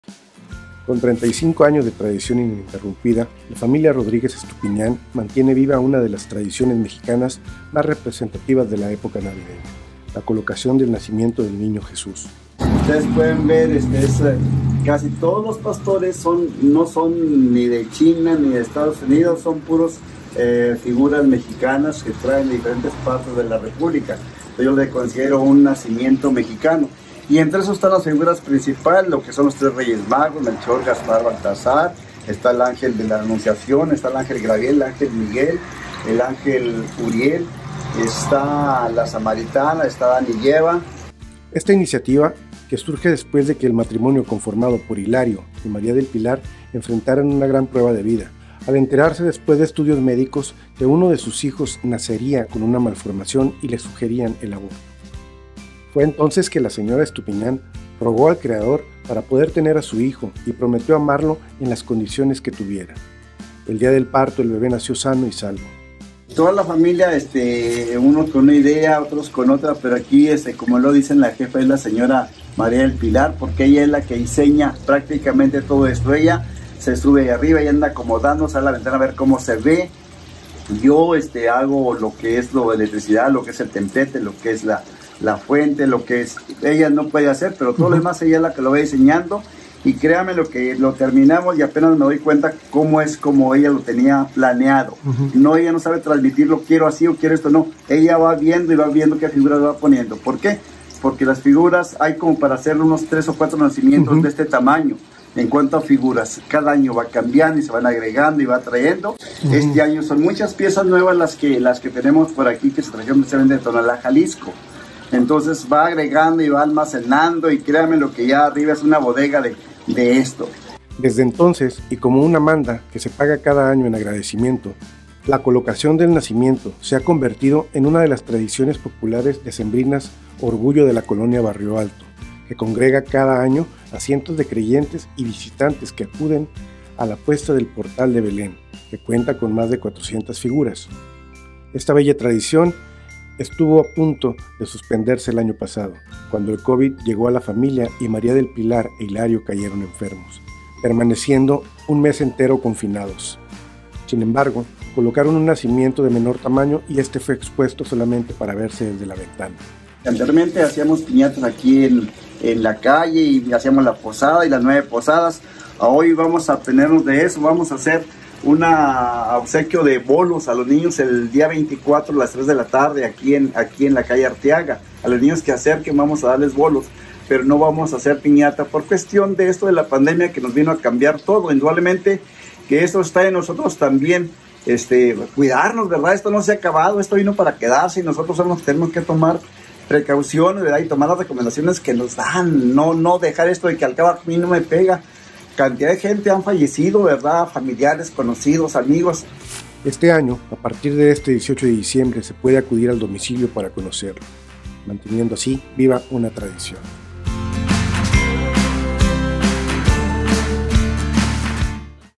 El nacimiento de Barrio Alto
Con 35 años de tradición ininterrumpida, la familia Rodríguez Estupiñán mantiene viva una de las tradiciones mexicanas más representativas de la época navideña, la colocación del nacimiento del Niño Jesús.